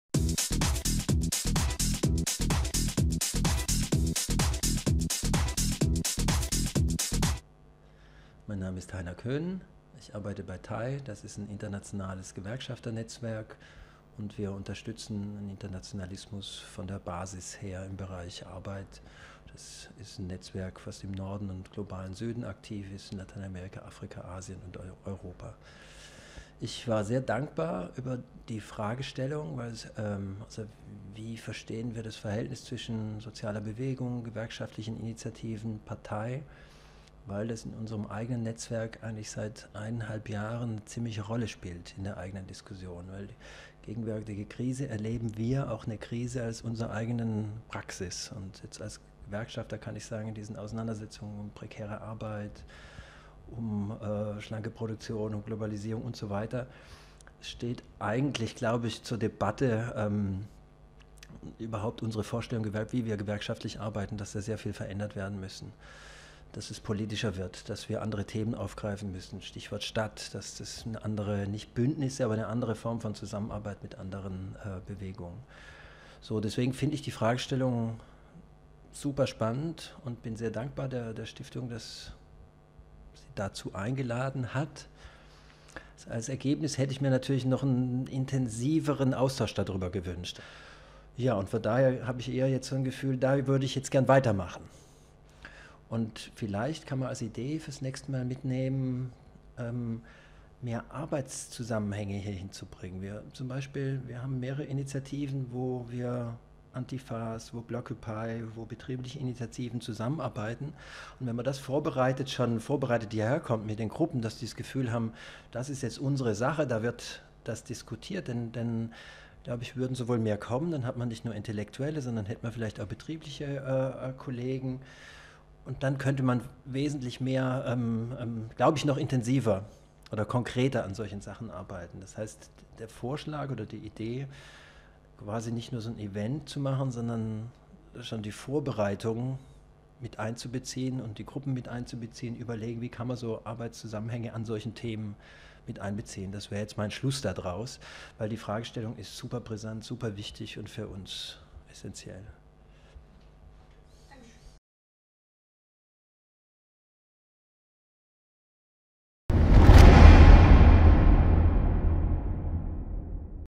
cross_solidarity II: Interview mit Heiner Köhnen
Dr. Heiner Köhnen ist wissenschaftlicher Publizist und seit Anfang der 90er Jahre freier Mitarbeiter des weltweiten Gewerkschafternetzwerks TIE Global Im Rahmen von TIE ist er Projektleiter mehrerer internationaler Projekte zu Gesundheit sowie zur weltweiten Automobil- und Bekleidungsindustrie in Europa, Afrika, Asien und Lateinamerika.

Kamera/Schnitt: Hans-Dieter Hey (r-mediabase.eu)
Interview: Susanne Kleinfeld